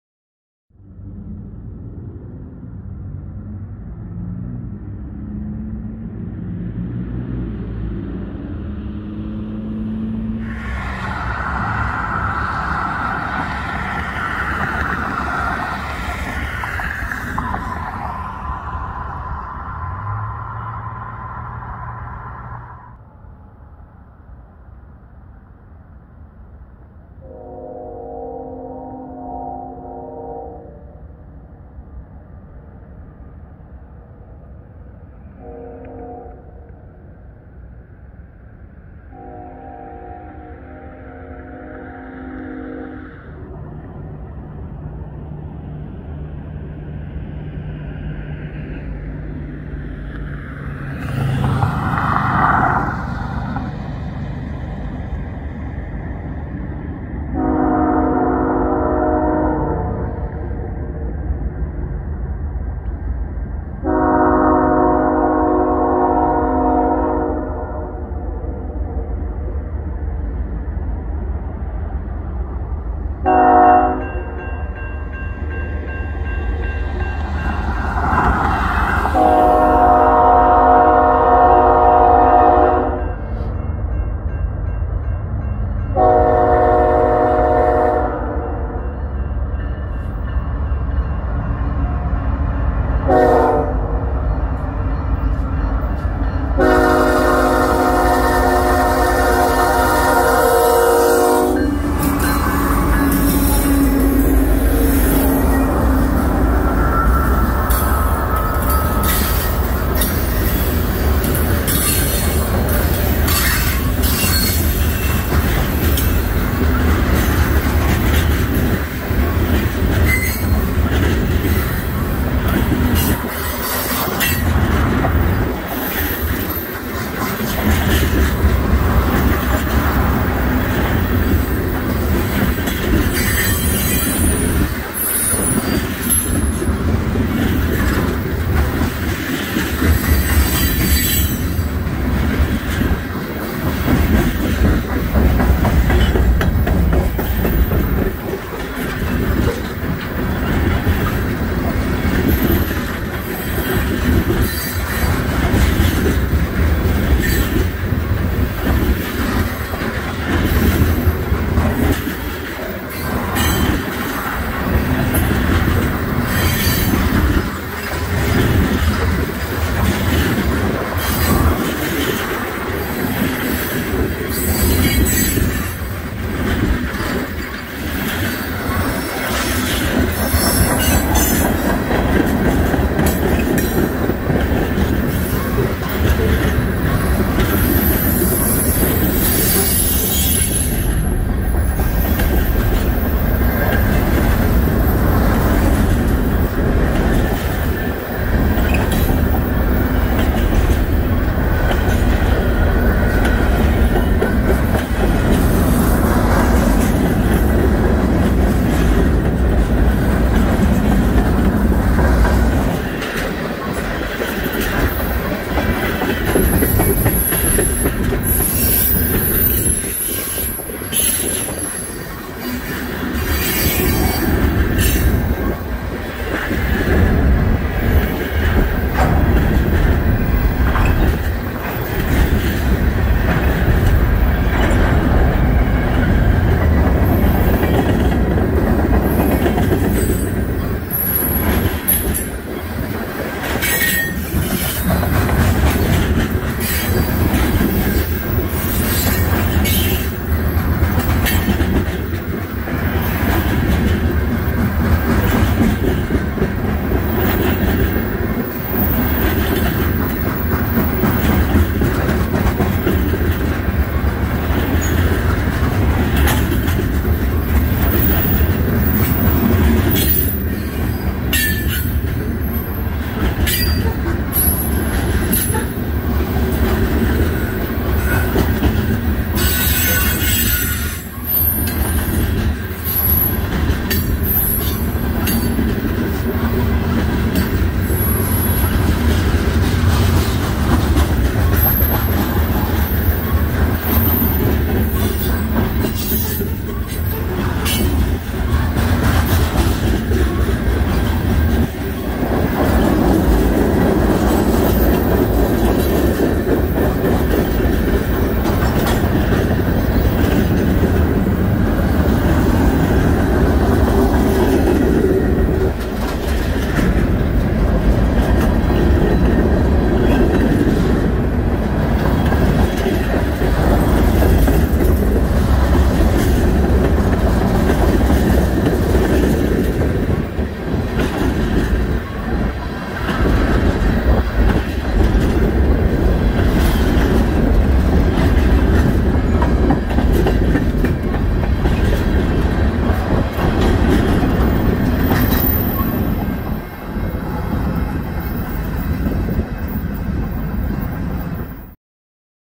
Canadian National M338 in Rockford, IL, 2022-03-27
M338 passes Morgan St. in Rockford near 21:30 with BNSF, CN, and KCS leaders.